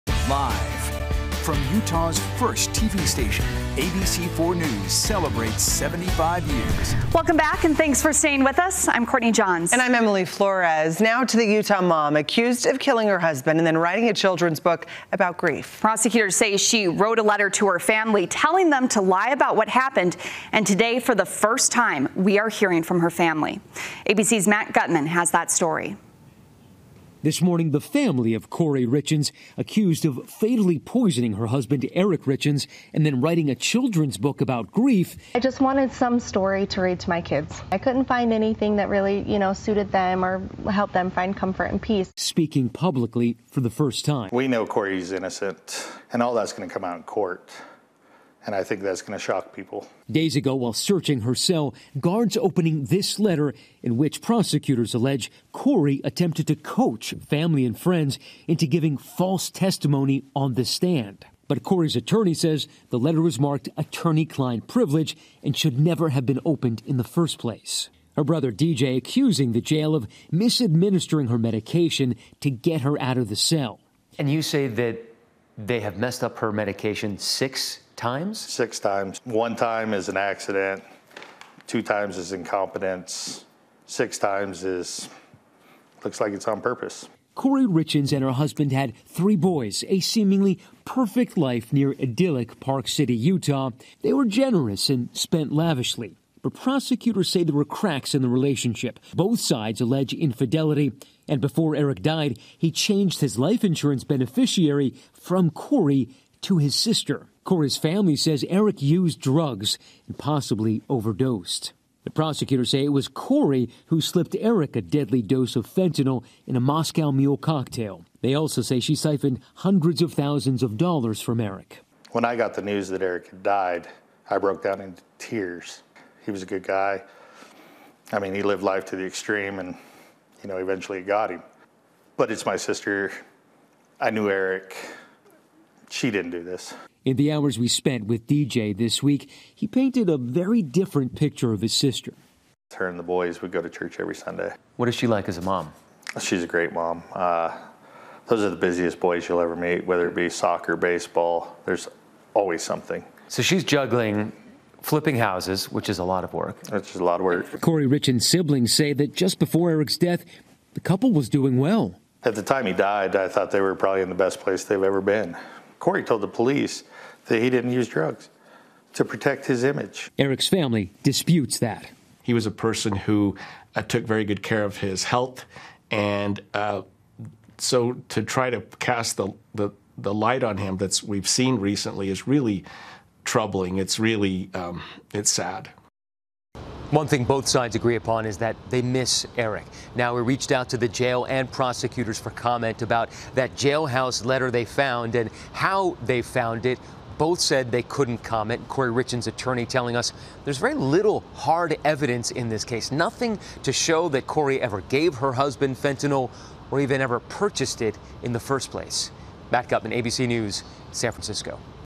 Kouri Richins' family speaks on allegations publicly for the first time
The family of Kouri Richins, the woman accused of killing her husband and writing a children's book about grief, speak publicly for the first time.